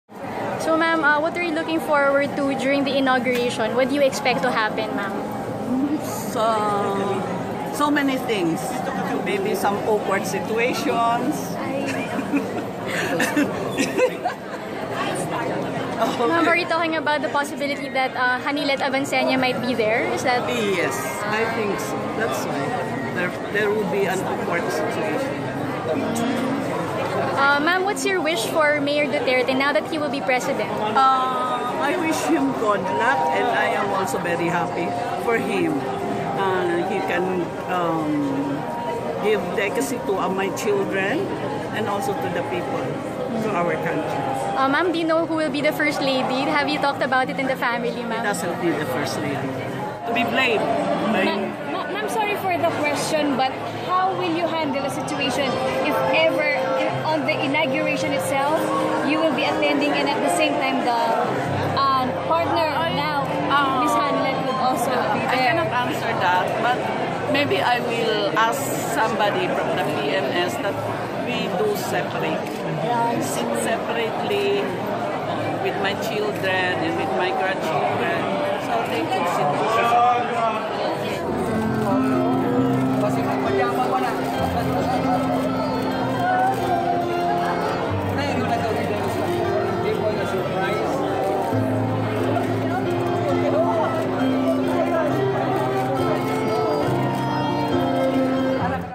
Duterte's ex-wife 'apprehensive' about inauguration
More videos on Rappler Follow Rappler for the latest news in the Philippines and across the globe. Elizabeth Zimmerman says she feels 'apprehensive' about encountering Duterte's current partner, Honeylet Avanceña, at his inauguration. Full story